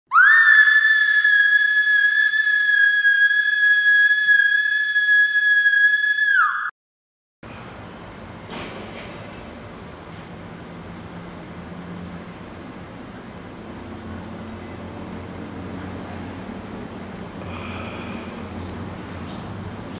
Teaser II: "El Grito del Nosotros" nuevas y viejas formas de alzar la voz
El Grito del Nosotros: ciclo de talleres para el activismo no violento del 10 al 12 de mayo en Caracas.